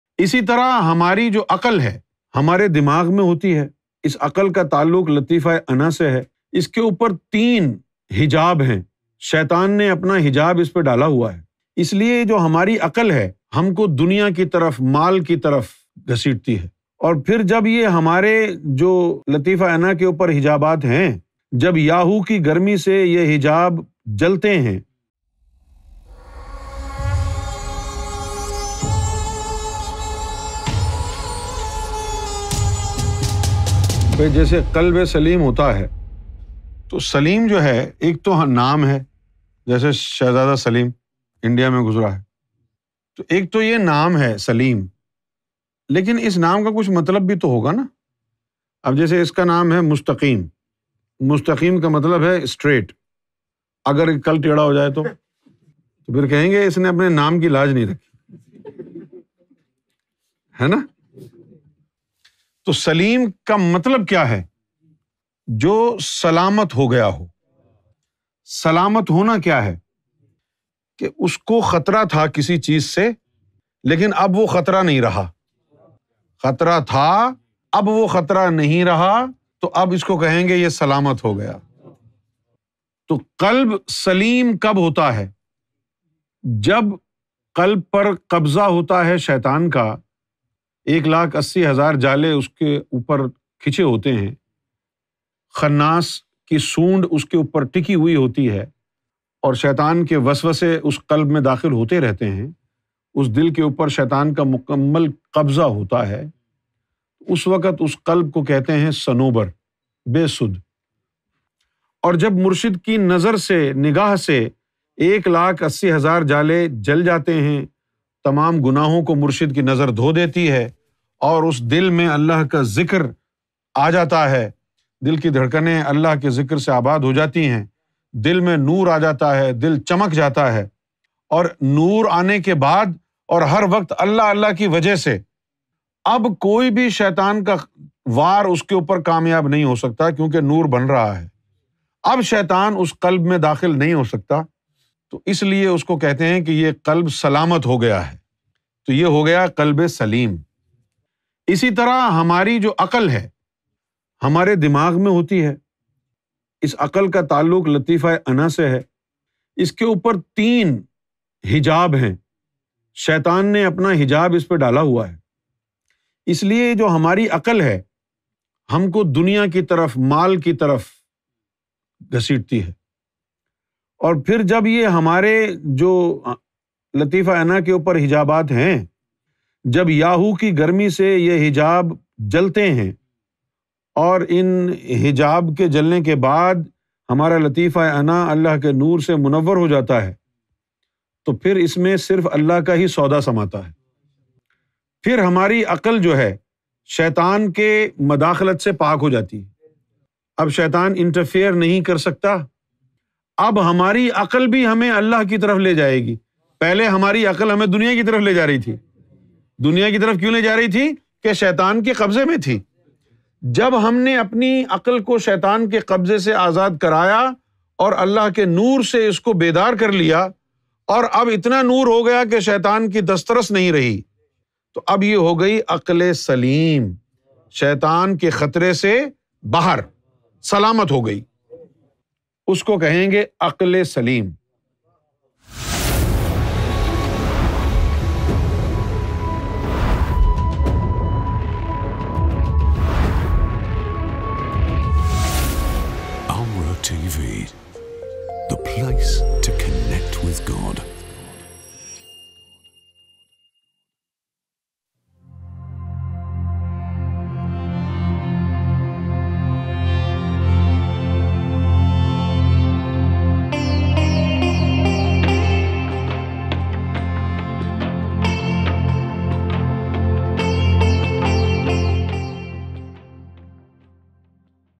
Aqal e Saleem Kya Hai? | Younus AlGohar | ALRA TV
Sufi Master Younus AlGohar provides the definition of Qalb e Saleem and Aqal e Saleem.